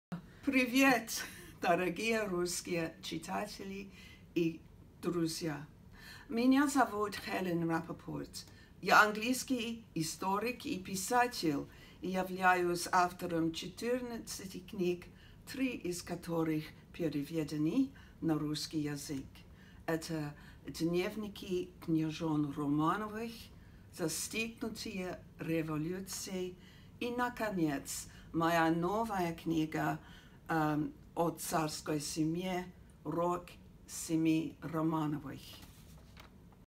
Раппапорт, Хелен. Рок семьи Романовых. «Мы не хотим и не можем бежать...»
Новая книга от автора бестселлеров «Дневники княжон Романовых» и «Застигнутые революцией» посвящена самой неизвестной странице жизни последнего российского императора — попыткам спасти от гибели Николая II и его семью.
Историческое расследование, основанное на недавно обнаруженных архивных материалах из России, США, Испании и Великобритании, прежде недоступных даже отечественным историкам, тщательно восстанавливает драматические события весны и лета 1917 года.
Венценосные европейские родственники Романовых и матросы-большевики, русские монархисты и британские разведчики — всем им история отвела свою роль в судьбе российской царской династии. Цепочка роковых совпадений переплетается с амбициями авантюристов, циничный политический расчет сталкивается с наивными надеждами, предательство маскируется под беззаветную верность…
Хелен Раппапорт виртуозно разбирает все запутанные версии и отвечает на главные вопросы – возможно ли было спасти царскую семью, и почему этого не произошло.